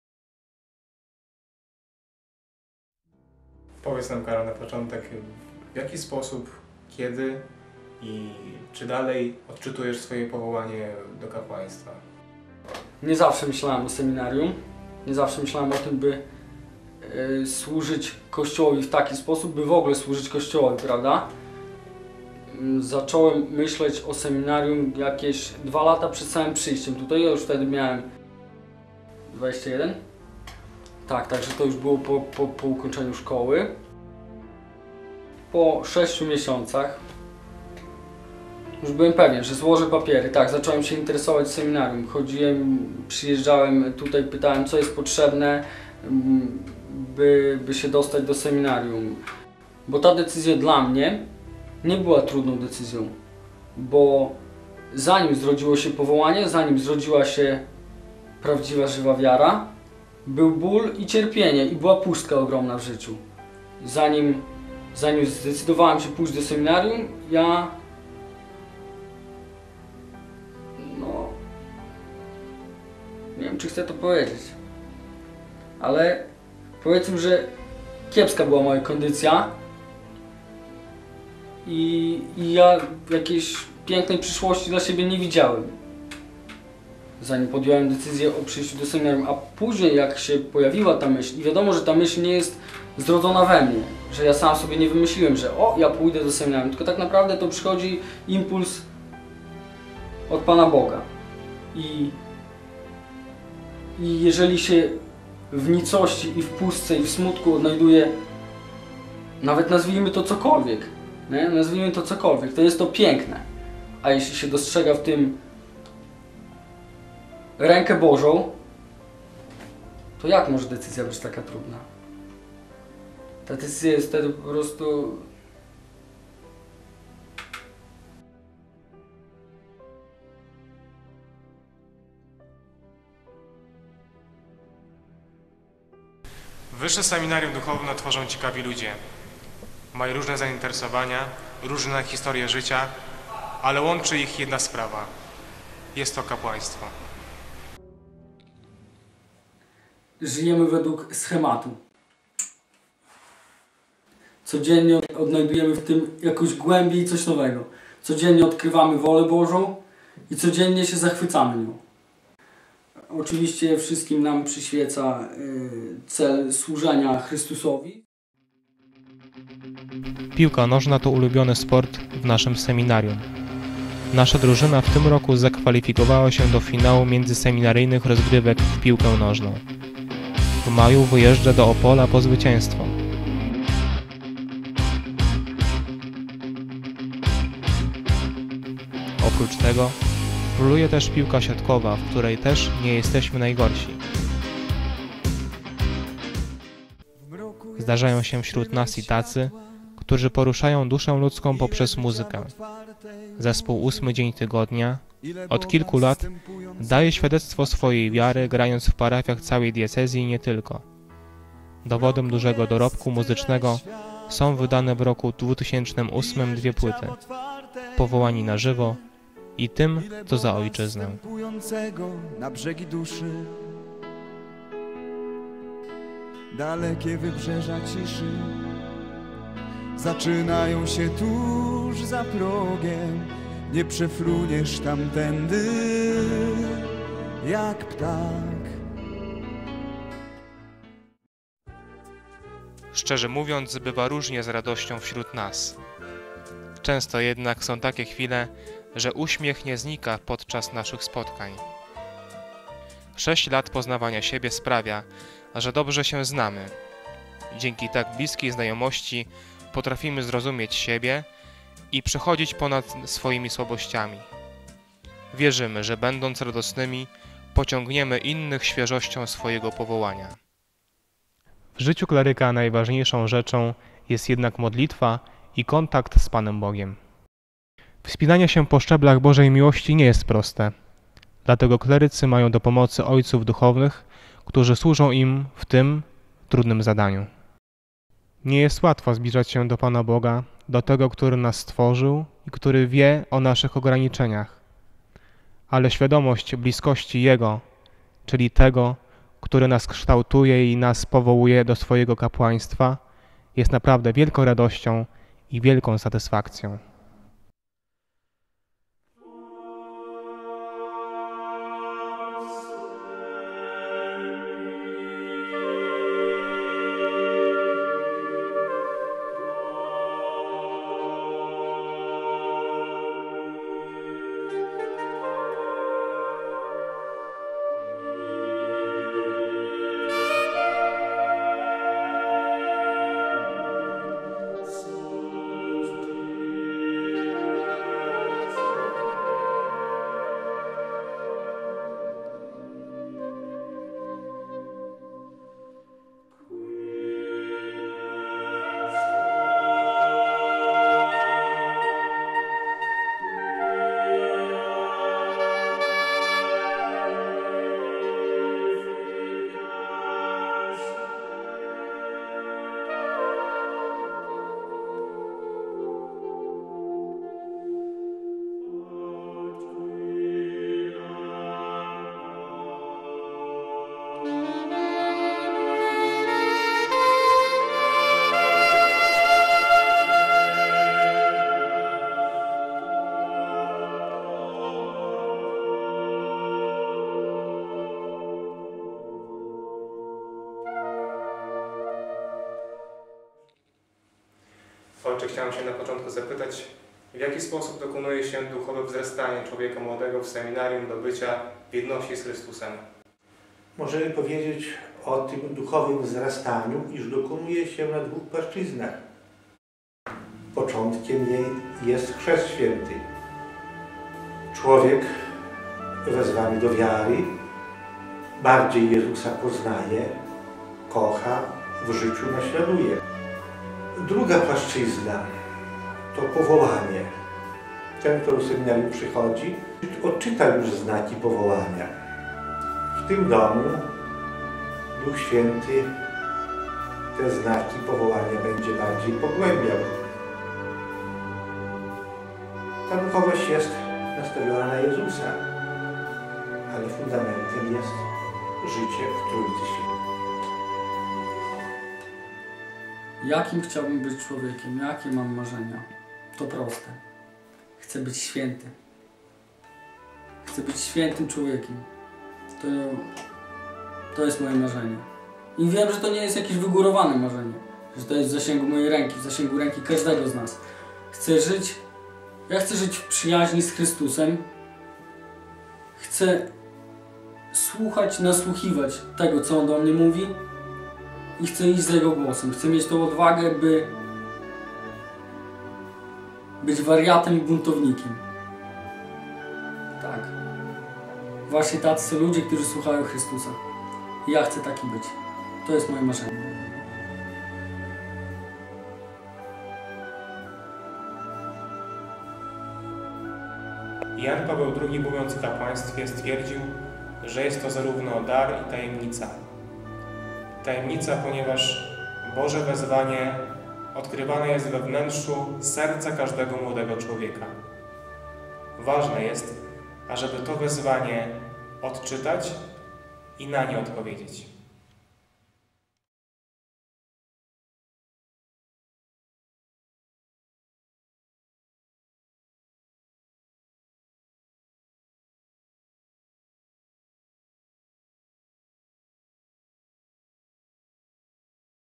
Film powołaniowy - WSD Łomża.mpg
Film opracowany przez kleryków Wyższego Seminarium Duchownego w Łomży o powołaniu do służby Bogu, czyli o kapłaństwie.
Prawa autorskie udostępnione przez reżyserów ;)